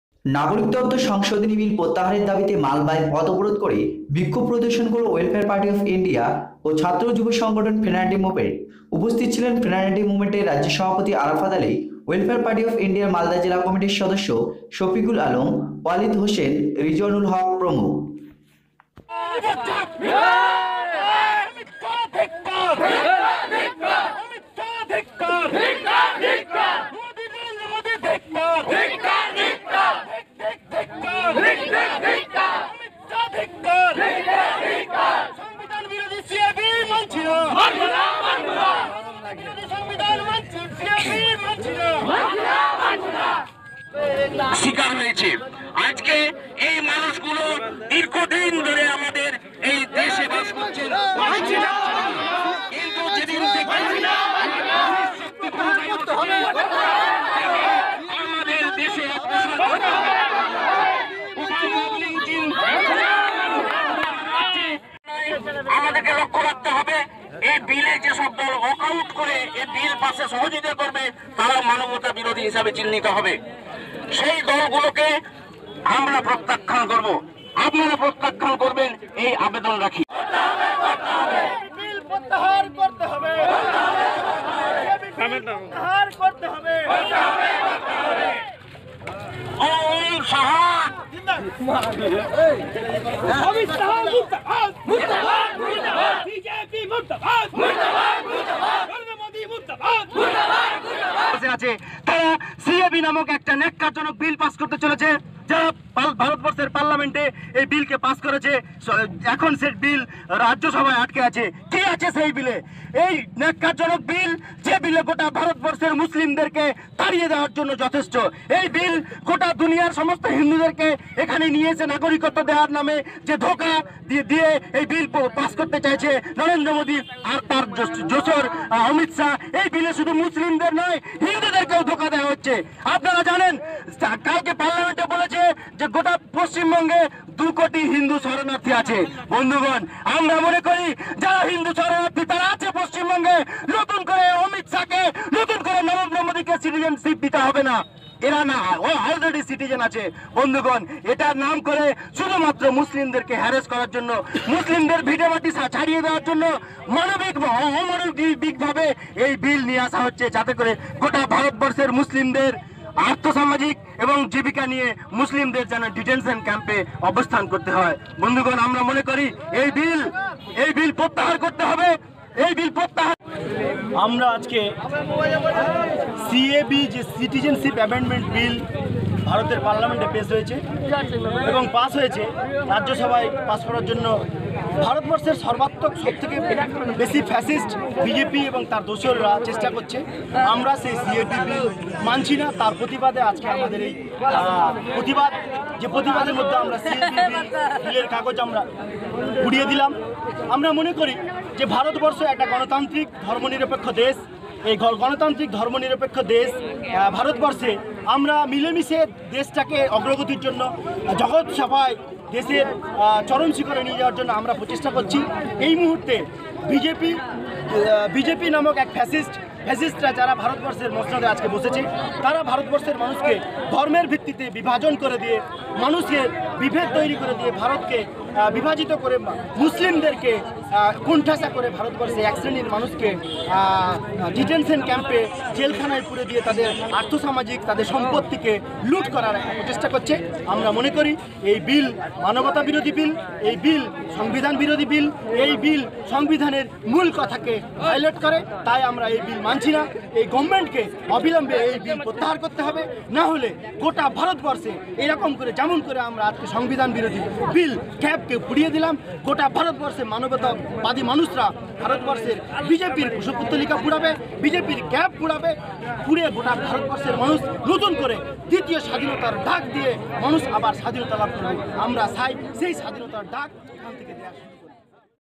নাগরিকত্ব সংশোধনী বিল প্রত্যাহারের দাবিতে মালদায় পথ অবরোধ করে বিক্ষোভ প্রদর্শন করলো WELFARE PARTY
নাগরিকত্ব সংশোধনী বিল প্রত্যাহারের দাবিতে মালদায় পথ অবরোধ করে বিক্ষোভ প্রদর্শন করলো ওয়েলফেয়ার পার্টি